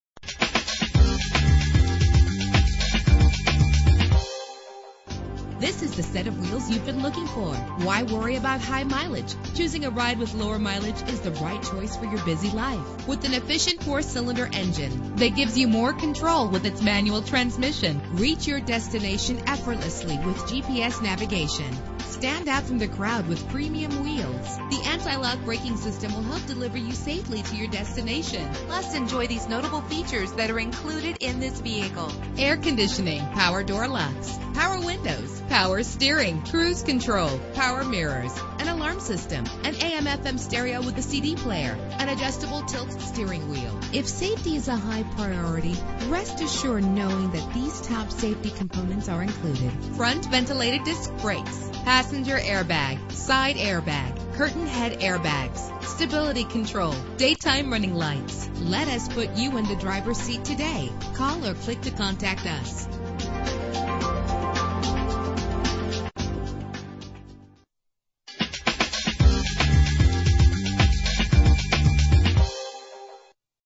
2011 Ford Fiesta - Owosso MI
Year: 2011
Make: Ford
Model: Fiesta
Trim: SES
Engine: 1.6 liter inline 4 cylinder 16 valve
Transmission: Manual
Color: Blue Flame Metallic
Mileage: 6052
Address: